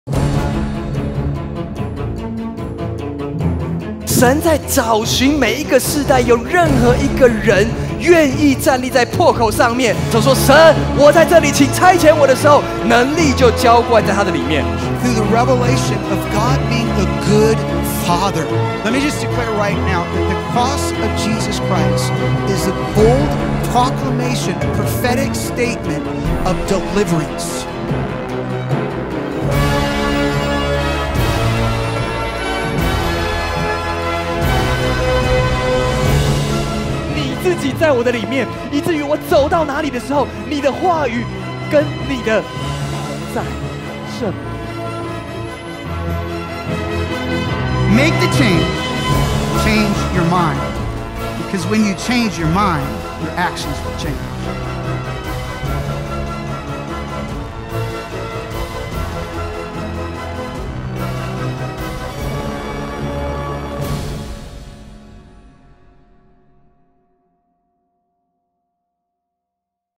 2016青年覺醒特會佈道會
青年佈道晚會

8/24(三) 19:00~21:30 克理斯・歐文斯特牧師
8/25(四) 19:00~21:30 晏信中牧師─「神國降臨醫治佈道會4」
(泰瑞莎‧戴蒙牧師團隊將以超自然藝術配搭服事)